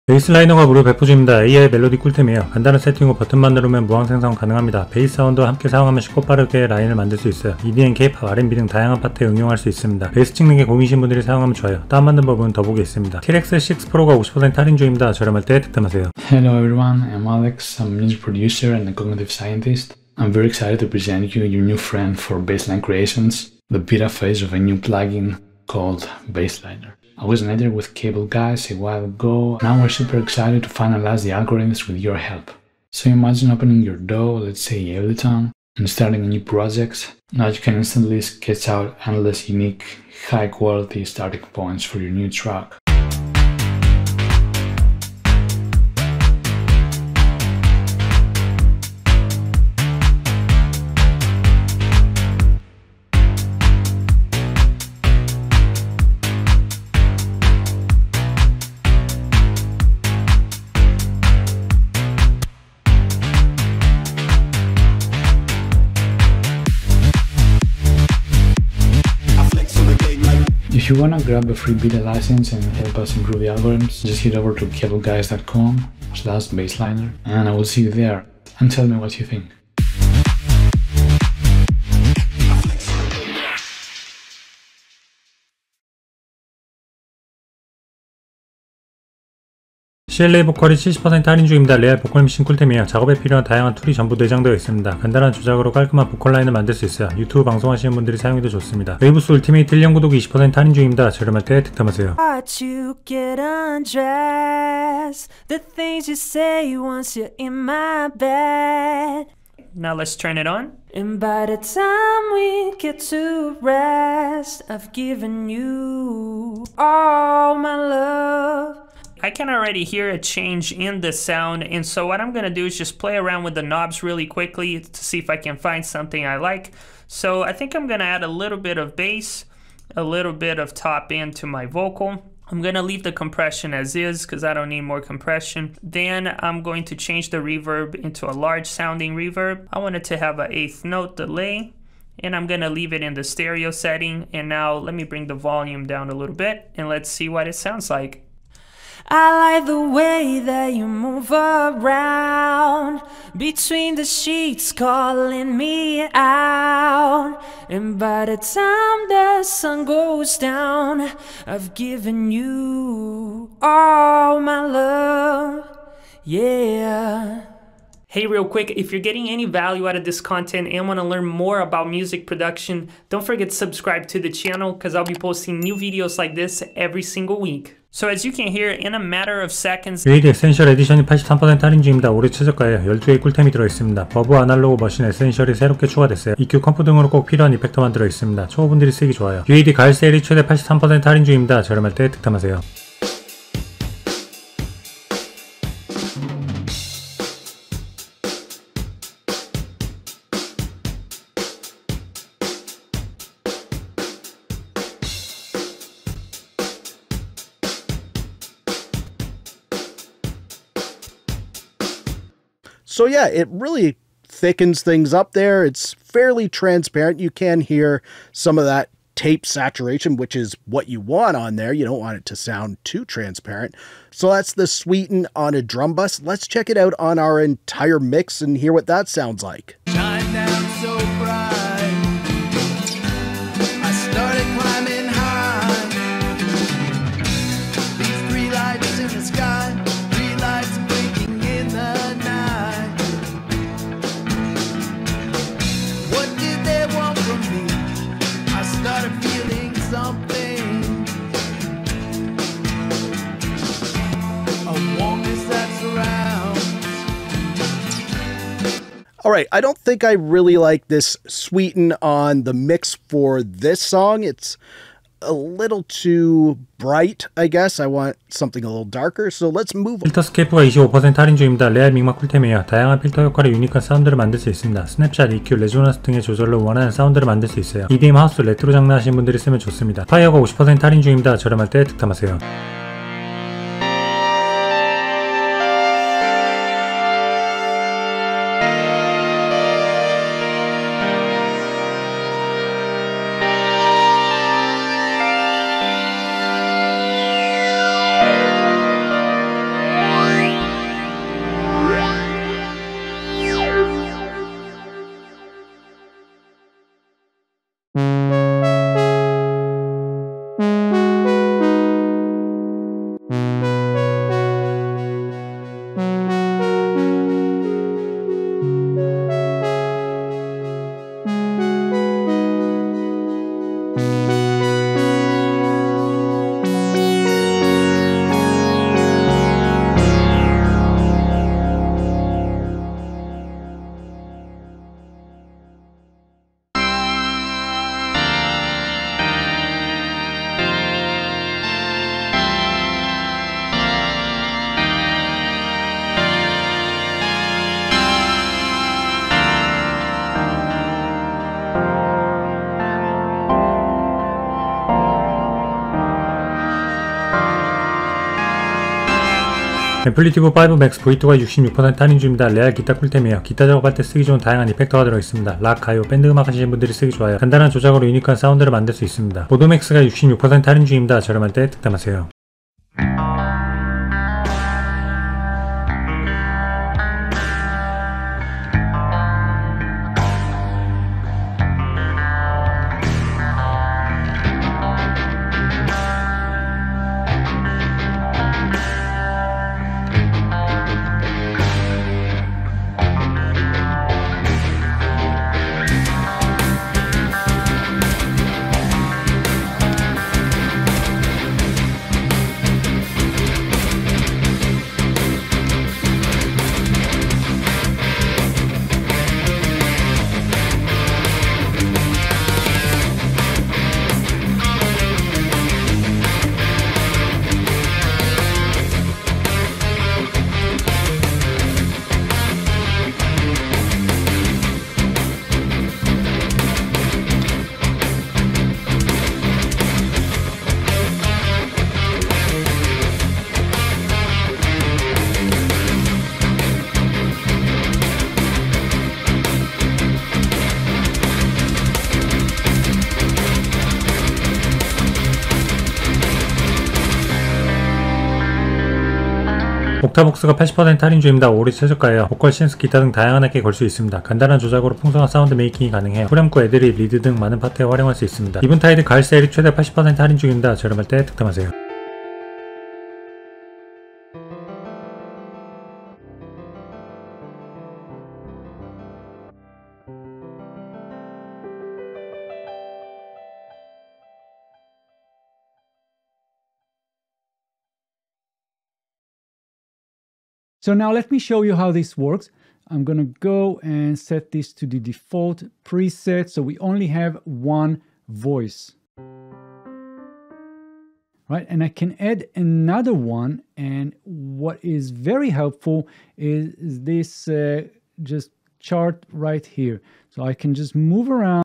안씀 손해
2024 가을 할인 모음

00:00 Bassliner (무료)

다운 받는 법
1. 사이트 하단 이메일 입력

01:30 CLA Vocals (70% 할인)

WAVES 울티메이트 1년 구독 (20% 할인) - 7일 무료 + 꿀템 230개 이상

WAVES 에센셜 1년 구독 (20% 할인) - 7일 무료 + 꿀템 110개 이상

03:00 UAD Essentials Edition (83% 할인) - 올해 최저가, 12개 꿀템

UAD 가을 세일 (최대 83% 할인) - UA 오인페 X

04:30 Filterscape (25% 할인)

06:00 AmpliTube 5 MAX v2 (66% 할인)

MODO MAX (66% 할인) - 모도 베이스 2 포함

07:30 Octavox (80% 할인) - 올해 최저가